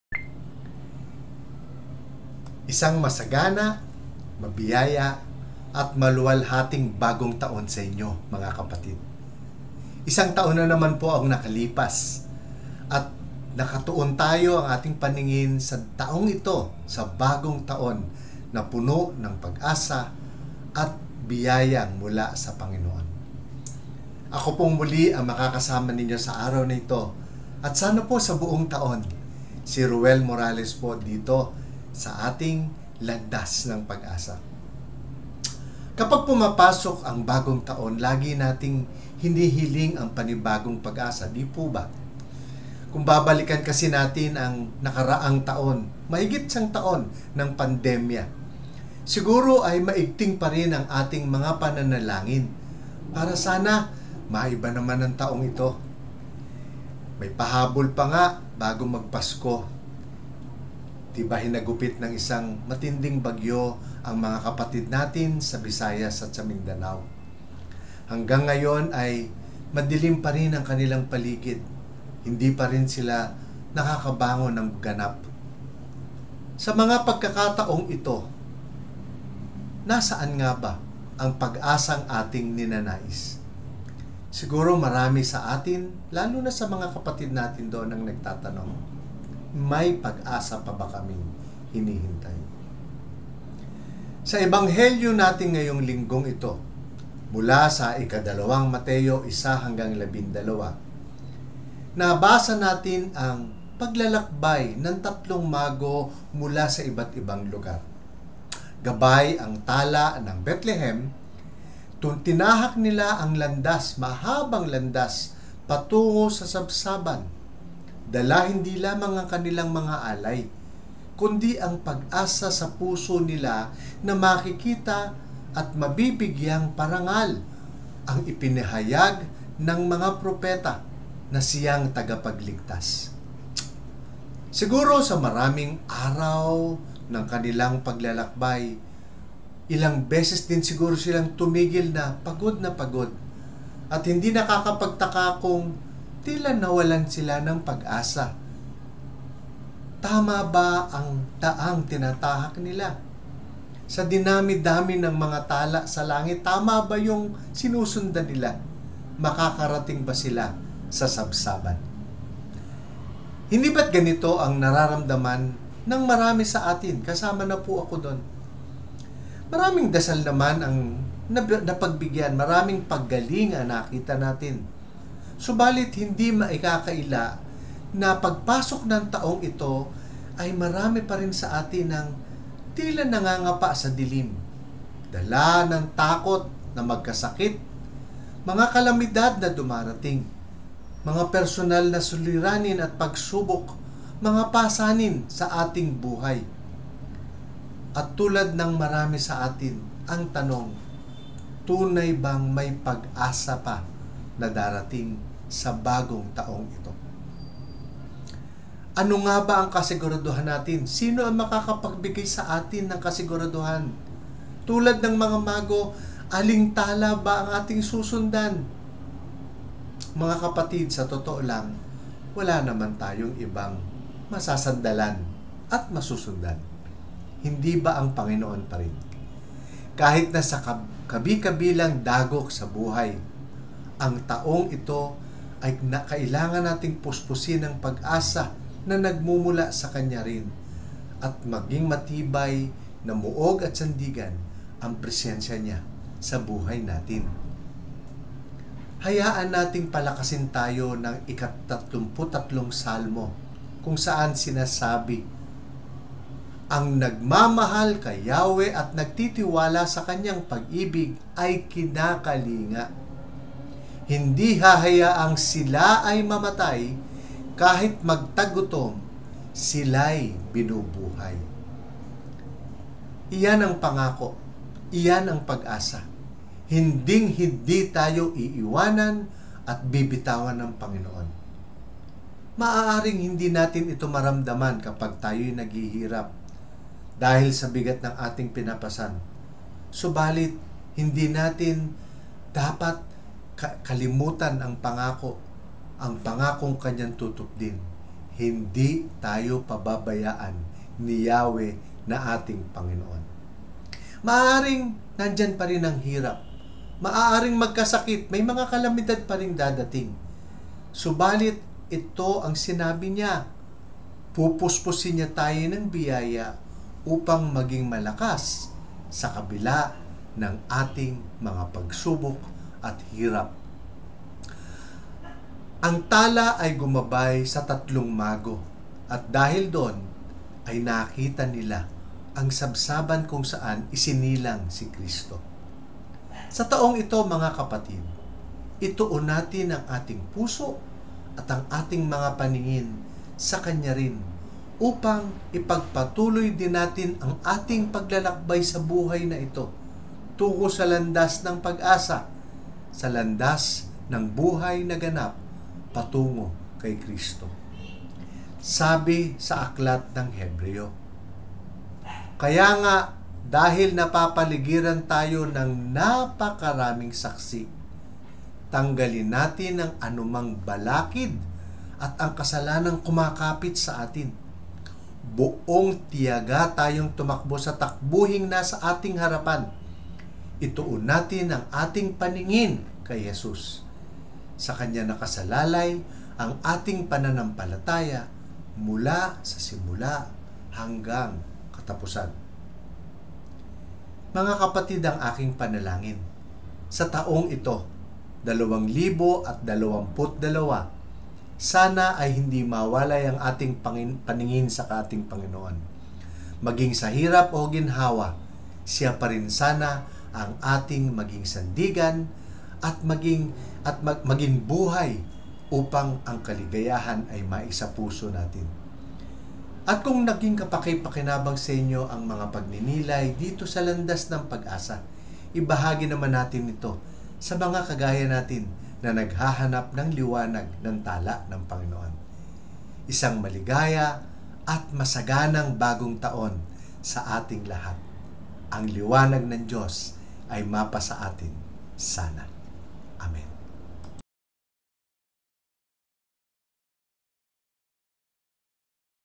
LANDAS NG PAG-ASA: “Panibagong Pag-asa”
Linggo, Enero 2, 2022
Dakilang Kapistahan ng Pagpapakita ng Panginoon

Si Hesus ay ipinanganak sa Betlehem ng Judea noong kapanahunan ni Haring Herodes. Dumating naman sa Jerusalem ang ilang Pantas mula sa Silangan at nagtanung-tanong doon: “Nasaan ang ipinanganak na Hari ng mga Judio? Nakita namin sa Silangan ang kanyang tala at naparito kami upang sambahin siya.”

Nang mabalitaan ito ni Haring Herodes, siya’y naligalig, gayun din ang buong Jerusalem. Kaya’t tinipon niya ang lahat ng punong saserdote at mga eskriba sa Israel at itinanong sa kanila kung saan ipinanganak ang Mesiyas. “Sa Betlehem po ng Judea,” tugon nila. “Ganito ang sinulat ng propeta:

‘At ikaw, Betlehem, sa lupain ng Juda,
ay hindi nga huli sa mga pangunahing bayan ng Juda.
Sapagkat sa iyo lilitaw ang isang pinuno
na mamamahala sa aking bayang Israel.’”

Nang mabatid ito, lihim na ipinatawag ni Herodes ang mga Pantas at itinanong kung kailan lumitaw ang mga tala. At pinalakad niya sila patungong Betlehem matapos pabilinan ng ganito: “Humayo kayo at inyong hanaping mabuti ang sanggol. Kapag inyong natagpuan, ibalita agad ninyo sa akin upang ako ma’y makasamba sa kanya.” At lumakad na nga ang mga Pantas. Muli silang pinangunahan ng talang nakita nila sa silangan hanggang sa sumapit ito sa tapat ng kinaroroonan ng bata. Gayun na lamang ang galak ng mga Pantas nang makita ang tala!

Pagpasok sa bahay, nakita nila ang bata sa piling ng kanyang inang si Maria. Lumapit sila at nagpatirapa at sinamba ang bata. Binuksan nila ang kanilang mga sisidlan at inihandog sa kanya ang dala nilang ginto, kamanyang at mira.

Nang sila’y pabalik na, sinabi sa kanila ng Diyos sa pamamagitan ng panaginip na huwag na silang babalik kay Herodes. Kaya, nag-iba na sila ng daan pauwi.

Pagninilay ni Ruel Morales : Unit Manager/Financial Adviser-AXA Phlippines. Independent Coffee Distributor. Covenanted Member-Ang Ligaya ng Panginoon